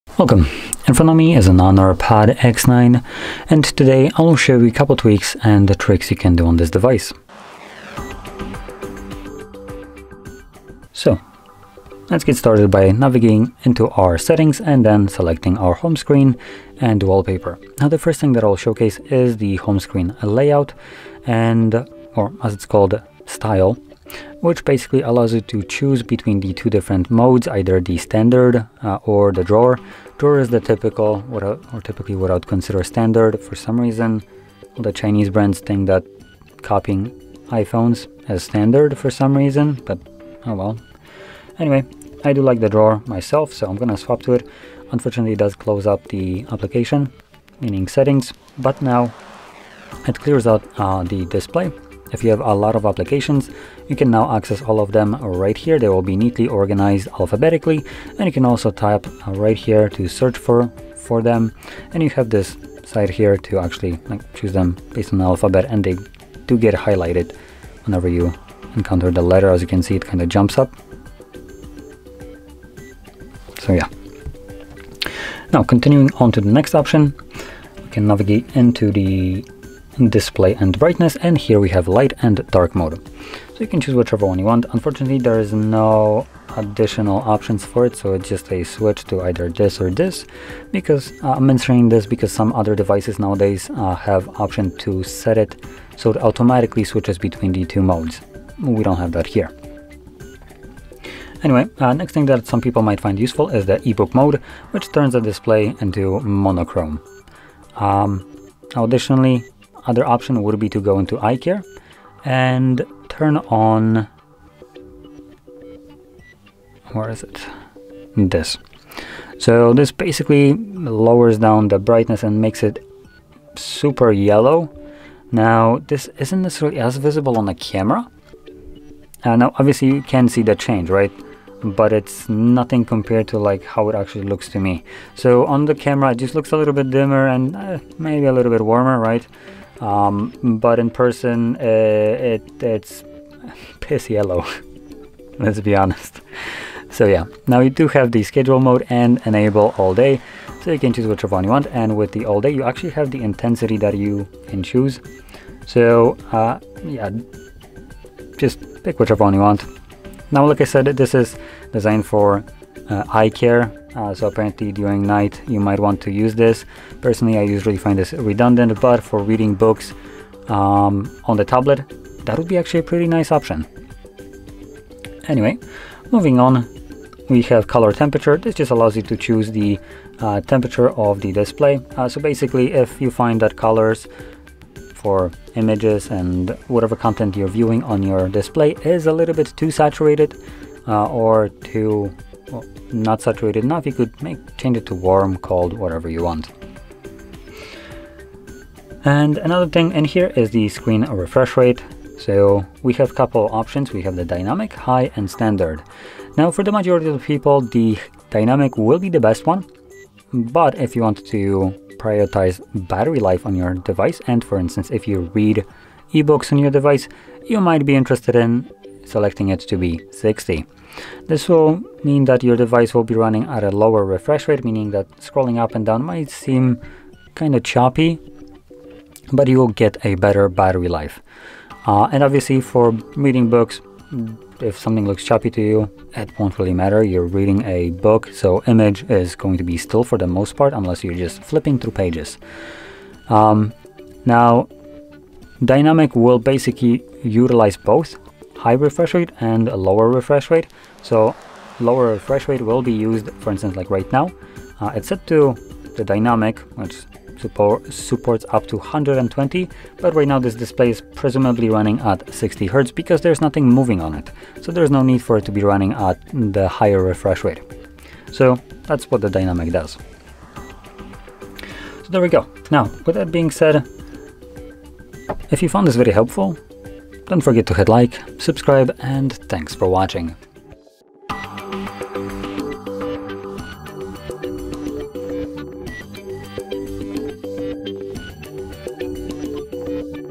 HONOR Pad X9 - Best Tricks and Tips - Most Useful Features and Settings
Unlock the full potential of your HONOR Pad X9 with our expert guide! In this video, we share the best tips and tricks to enhance your tablet experience. From productivity hacks to customization options, follow along for valuable insights and make the most out of your HONOR Pad X9. Dive into this collection of expert tips and elevate your tablet usage to new heights.

How to set up and use dark mode on HONOR Pad X9? How to change the display refresh rate on HONOR Pad X9? How to customize display colors on HONOR Pad X9?